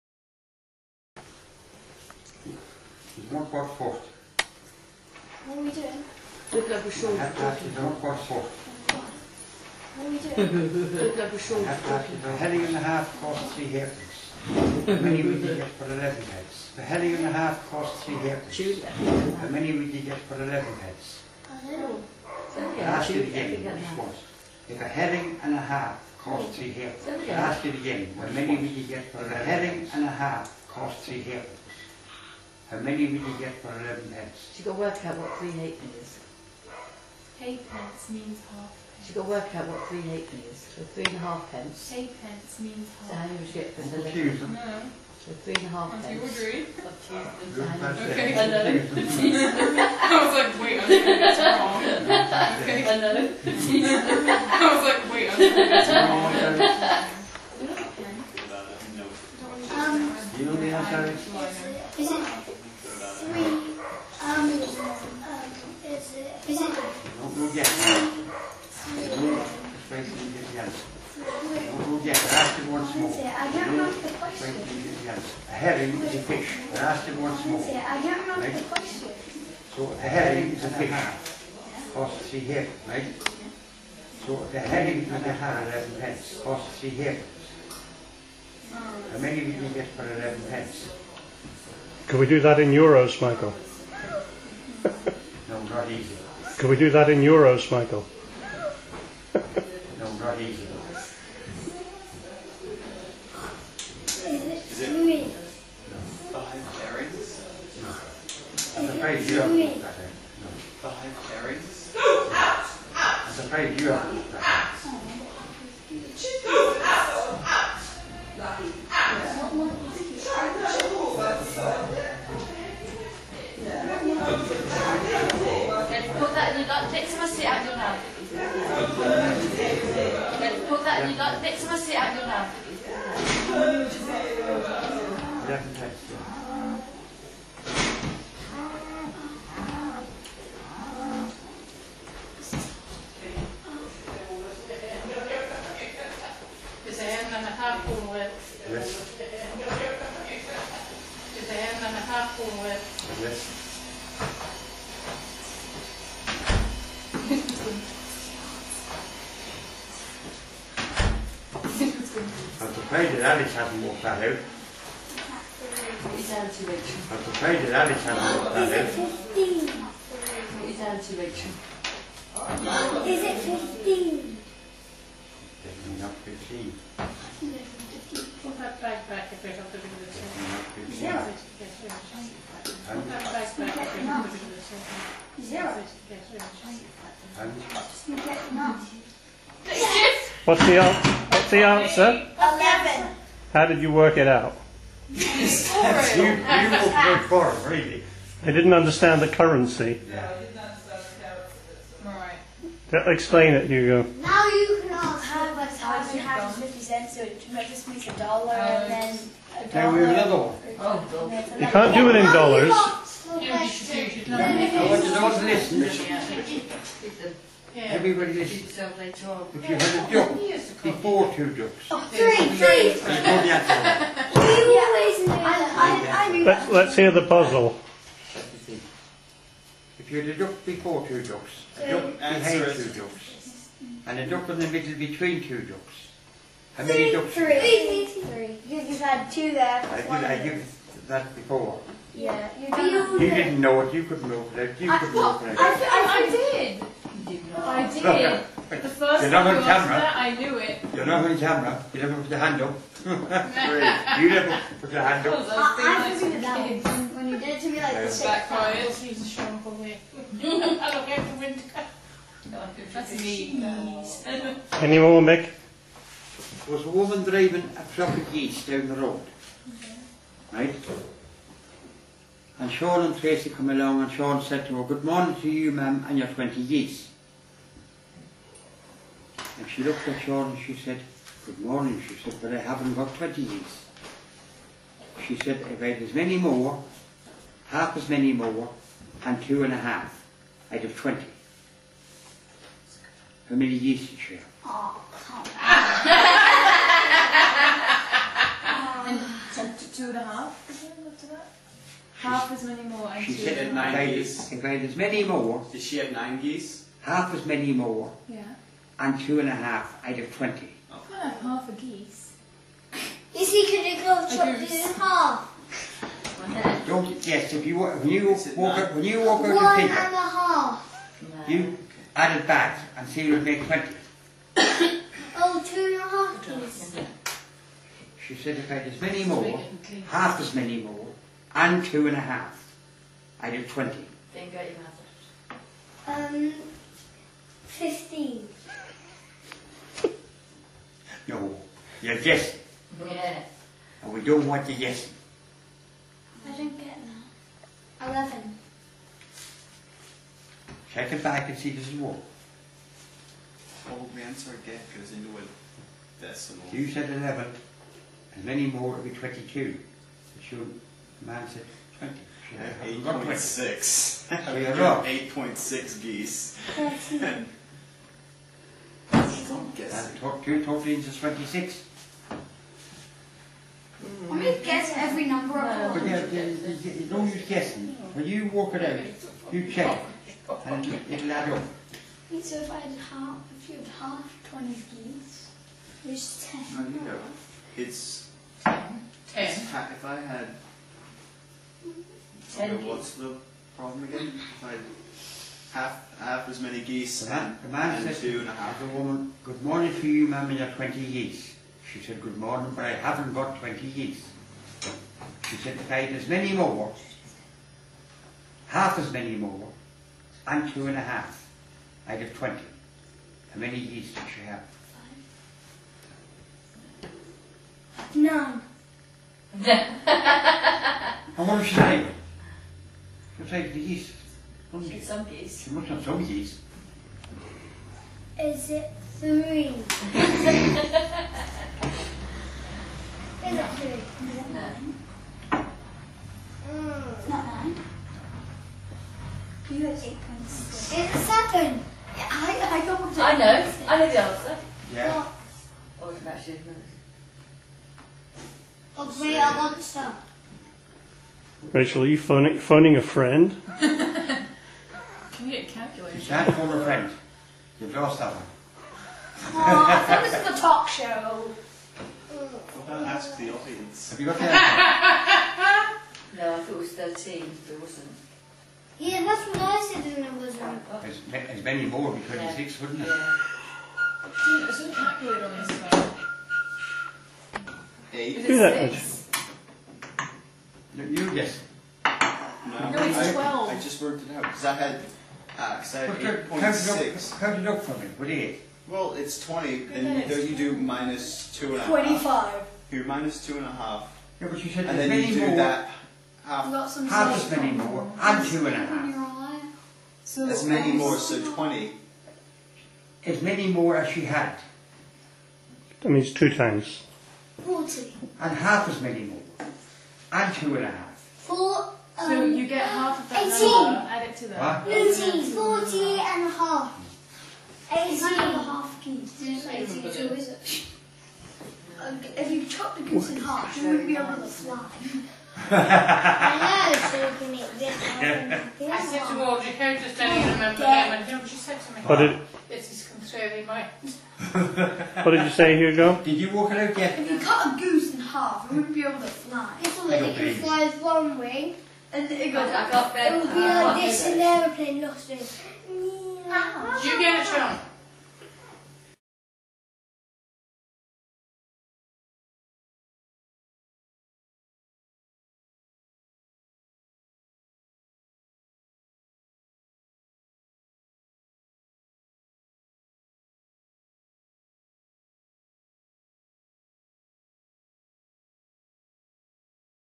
Grandad's Maths Puzzles for his Grandchildren
Michael Victory sets some brainteasers for his grandchildren